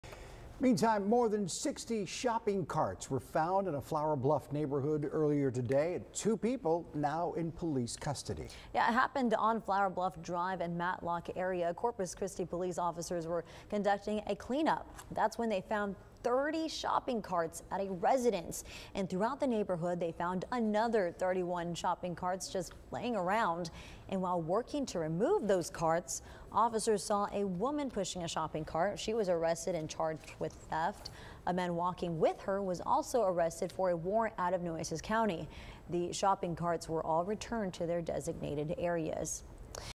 60+ stolen shopping carts recovered in Flour Bluff neighborhood, arrest made
31 shopping carts were found at one residence near Flour Bluff Drive and Matlock and 30 more were found on surrounding streets.